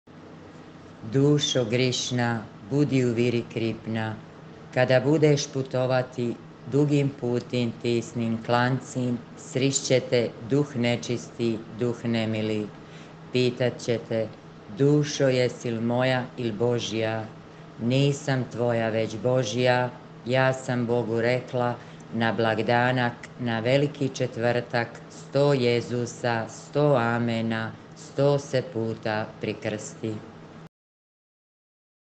Dušo grišna. Molitva na Veliki Četvrtak.
▶️Tonski snimak je od Vesne Budimir.
🔸️TEKST MOLITVE: O dušice grišna, budi u viri kripna! Kada budeš putovati dugim putim, tisnim klancim, srist će tebe duv nemili, duv nečisti. Pitat će te, je’l si dušo moja il Božija. Nisam tvoja, već Božja. Ja sam reka svome Bogu doći na blagdanak, na Veliki četvrtak, sto križića,  sto Amena, sto se puta prikrsti.